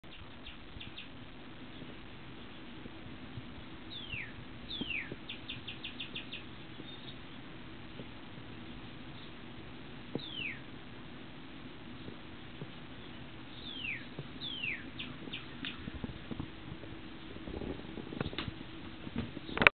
Cardinal sing.MOV
Crazy Cardinal singing in the Dogwood tree outside the sunroom